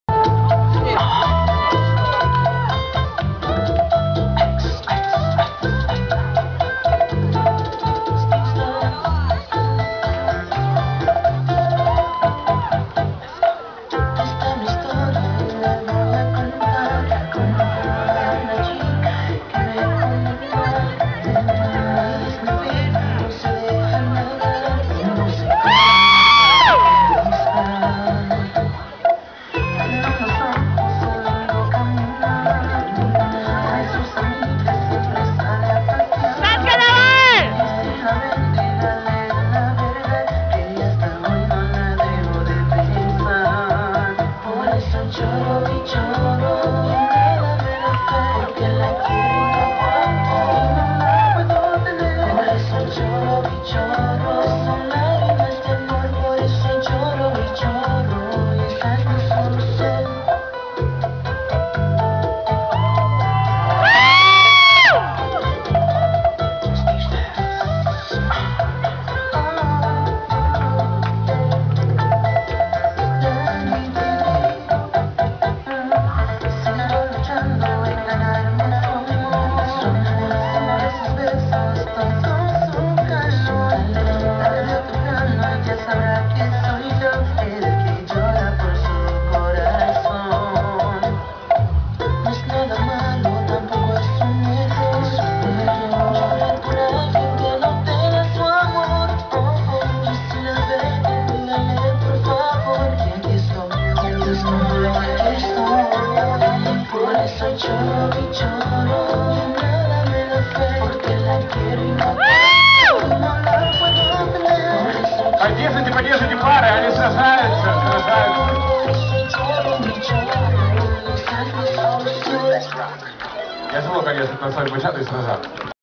Чемпионат "Salsa-Projeсt 2013" - Bachata Begginers - полуфинал перетанцовка
13 апреля 2013 г.
г.Донецк

Категория "Bachata Begginers" - полуфинал перетанцовка

№ 2 - Карандашов Дмитрий и Сычева Александра (ЛЕ Шкробтак)

Организатор: школа танцев "Salsa Project"